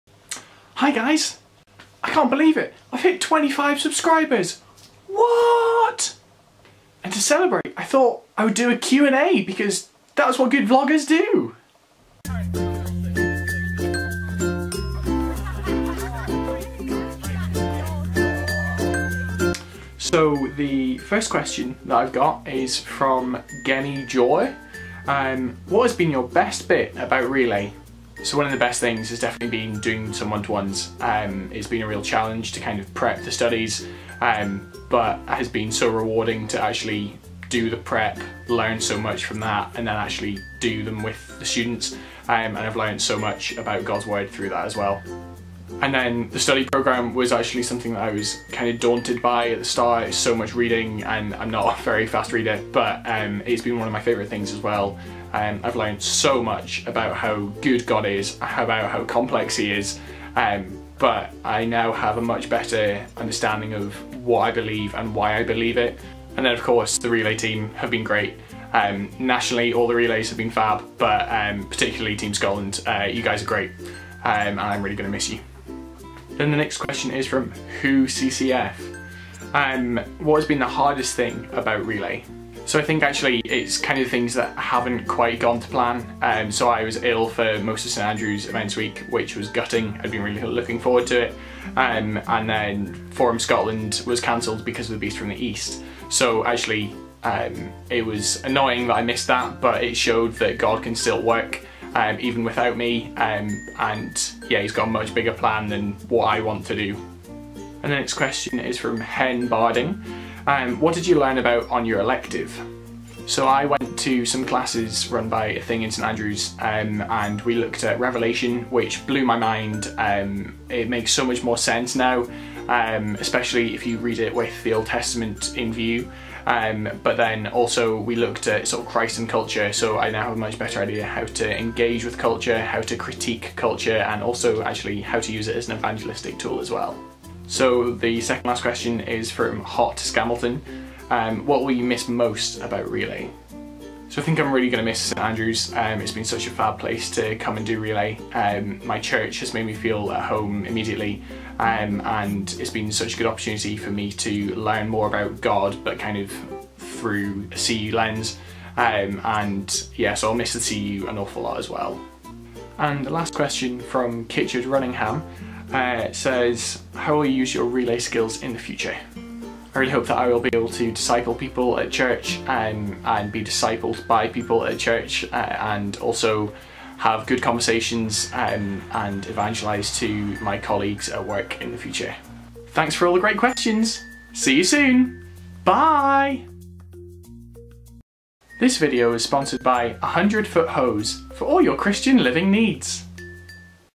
Relay Q&A // A Relay Vlog
So at the end of the year we had to make a wee presentation for our final conference all about our year.  Of course I thought it would be rude not to do something Vloggy so I did this wee video as a bit of a micky take of vlogger Q&A videos